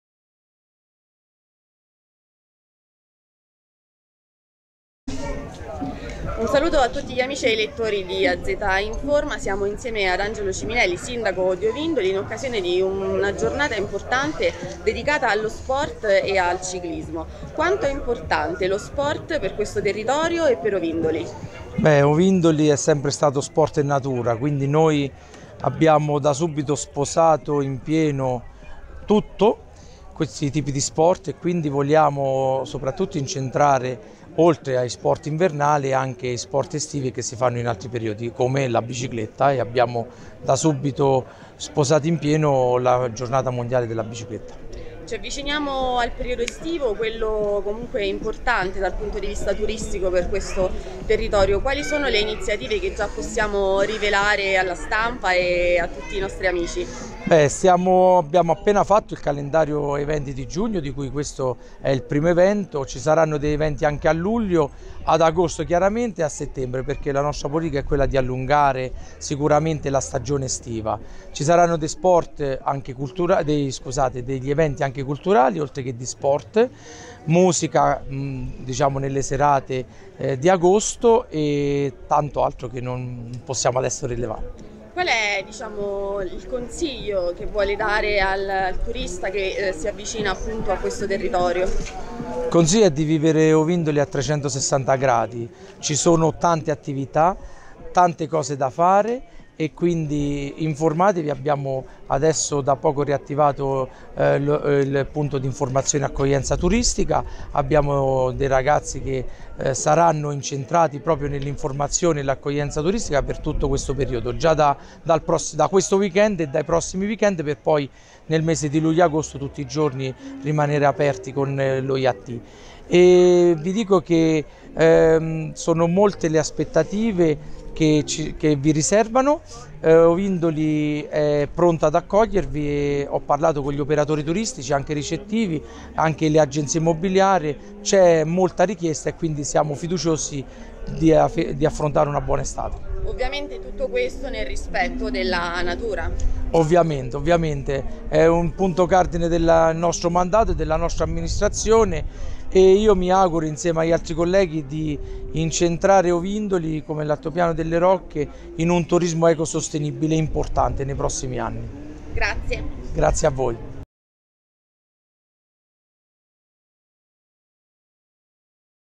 Intervista al sindaco di Ovindoli, Angelo Ciminelli
Sport veicolo di turismo e fondamentale per un territorio come quello di Ovindoli e dell’Altopiano delle Rocche. Ne abbiamo parlato con il sindaco Angelo Ciminelli

A cura di Antonella Valente